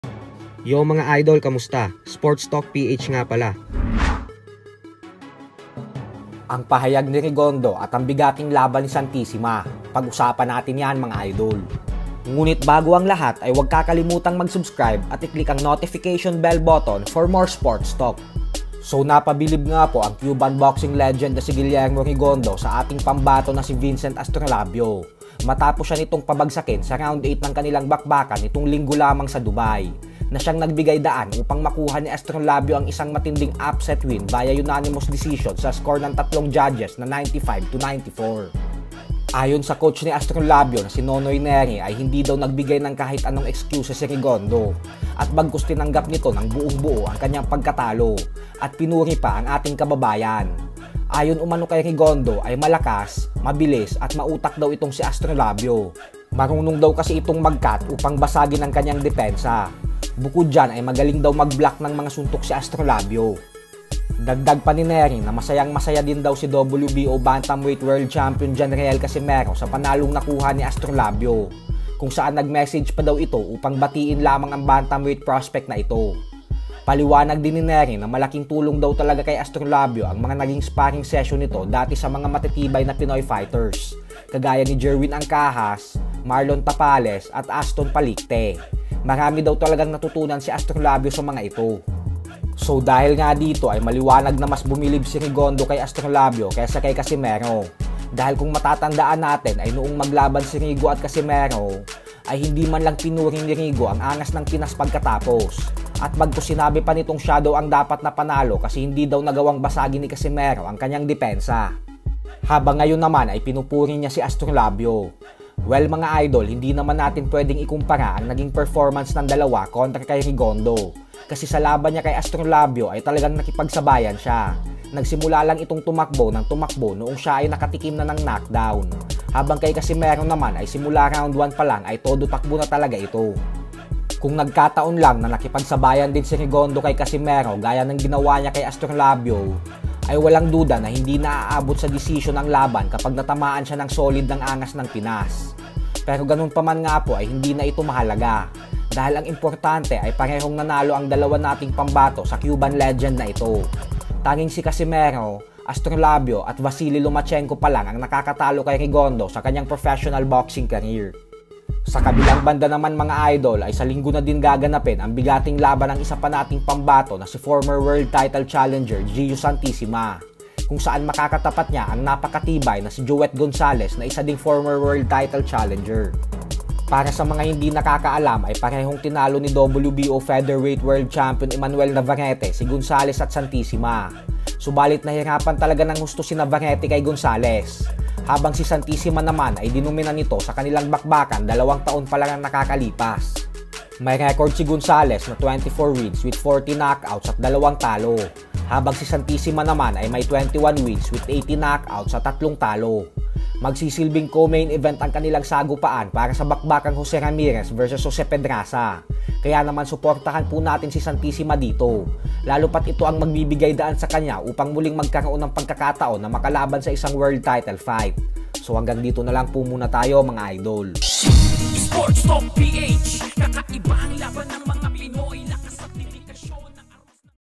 Rigo MAS bumilib kay Astrolabio kesa kay Casimero! | BIG FIGHT ni Santisima sa Linggo na
Guillermo Rigondeaux vs John Riel Casimero and Vincent Astrolabio | Jeo Santisima vs Joet Gonzalez | Boxing news & updates!